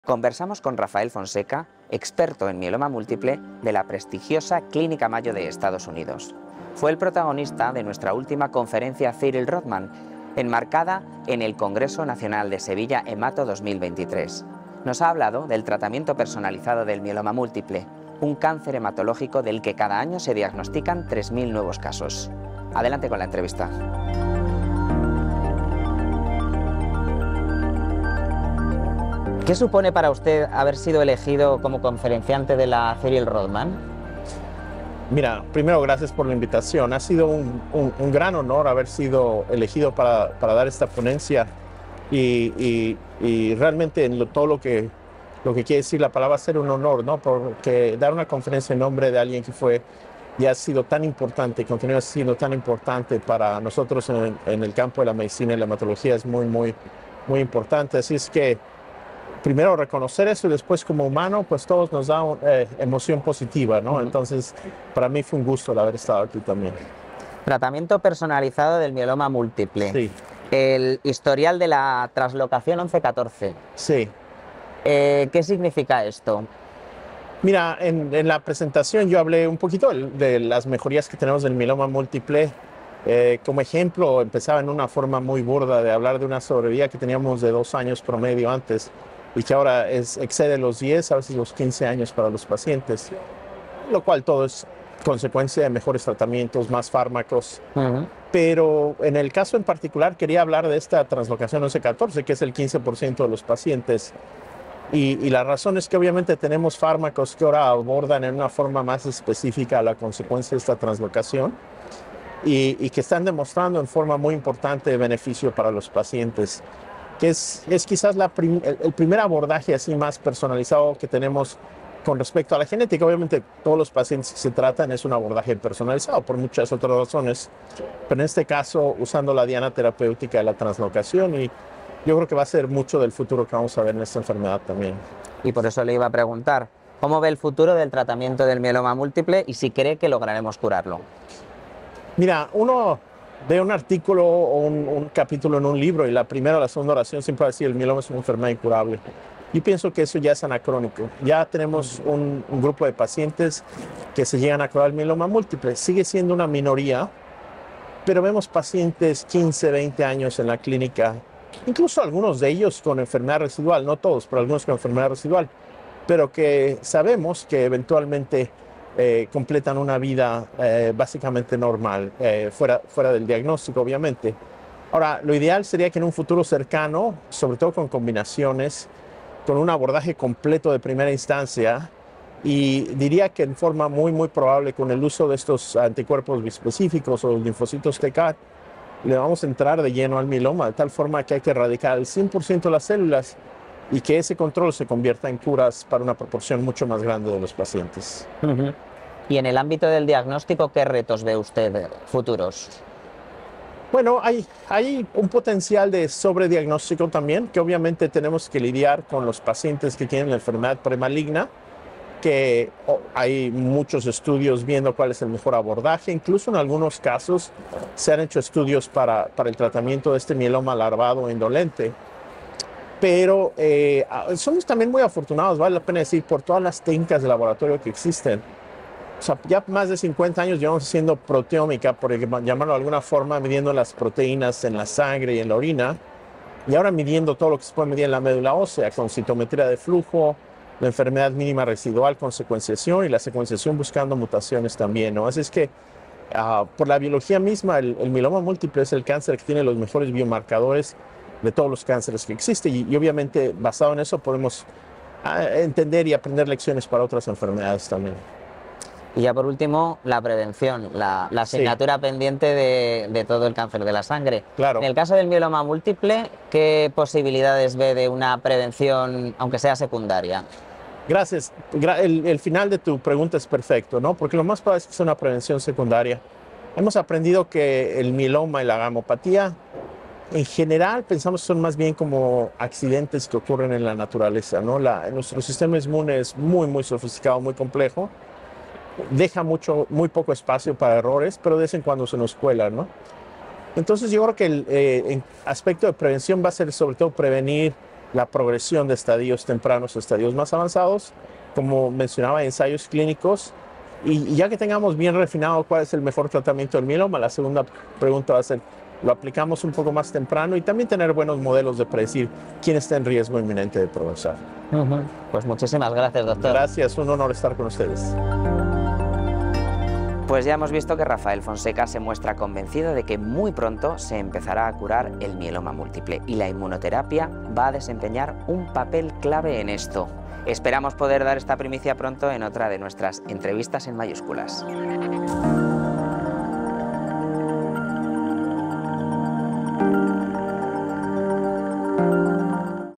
EntrevistasEnMayúsculas - Rafael Fonseca
Entrevistamos a Rafael Fonseca, experto en mieloma múltiple de la prestigiosa Clínica Mayo de Estados Unidos. Fue el protagonista de la V Conferencia "Ciril Rozman", enmarcada en el Congreso Nacional de Sevilla, Hemato2023. Nos ha hablado del tratamiento personalizado del mieloma múltiple, un cáncer de la sangre del que cada año se diagnostican 3.000 nuevos casos.